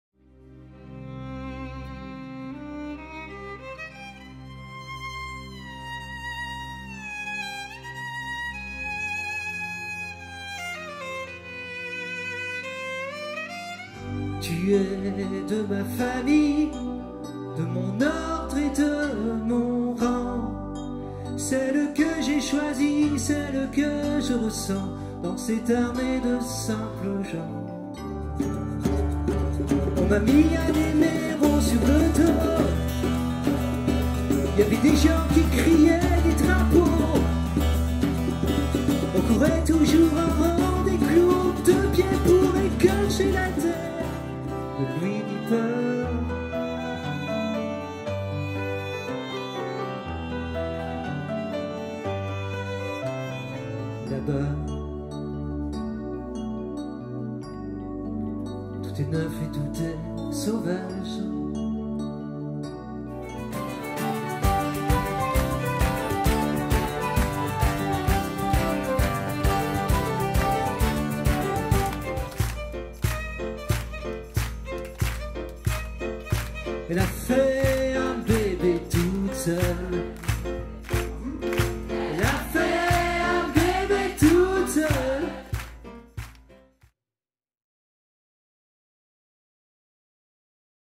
Teaser 2 Goldman Confidentiel - CoverGoldman Trio
« Goldman Confidentiel », c’est l’univers de Jean-Jacques Goldman dans une version acoustique totalement inédite !

Durant une heure trente, ce trio vous fera découvrir ou redécouvrir le répertoire du chanteur préféré des français, des grands standards aux morceaux plus intimistes grâce au savoir-faire de Christophe Celen, Christophe Genette et Madeleine Jacques.
Bien plus qu’un concert, ce trio vous plongera dans un spectacle INTERACTIF où le public du soir pourra contribuer et participer en sélectionnant les chansons interprétées !

À trois, ils chantent, ils jouent de plusieurs instruments, s’amusent et plaisantent avec le public. Celui-ci est charmé, épaté, il en redemande... Chaque représentation fait l’unanimité, même chez les fans les plus inconditionnels de l’artiste !

Prenez place, détendez-vous, on monte le son... Découvrez sans plus attendre le spectacle, sans doute le plus personnel, sur les chansons de Jean-Jacques Goldman... « Goldman Confidentiel », un moment exceptionnel en toute intimité !